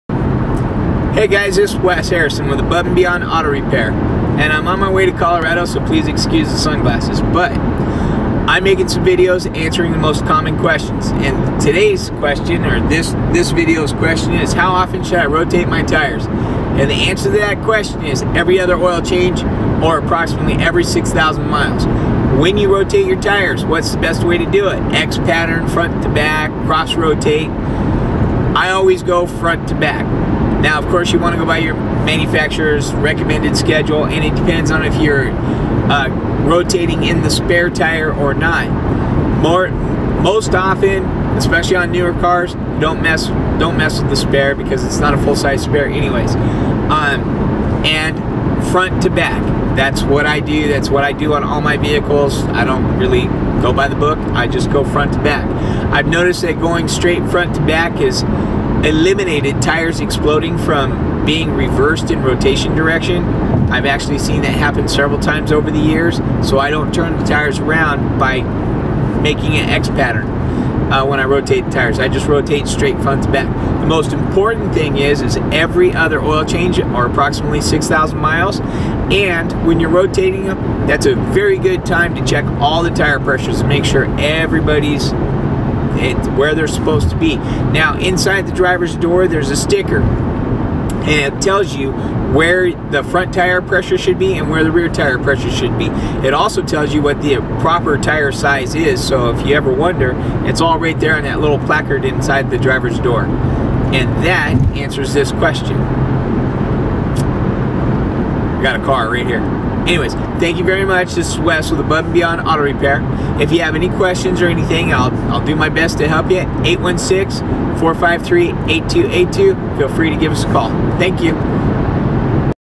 Best Way to Rotate Tires | Auto Repair | Kansas City | Car
Above and Beyond Auto Repair
Proudly Serving all the Greater Kansas City Metro Area

This video explains what I feel is the best way to rotate tires on vehicles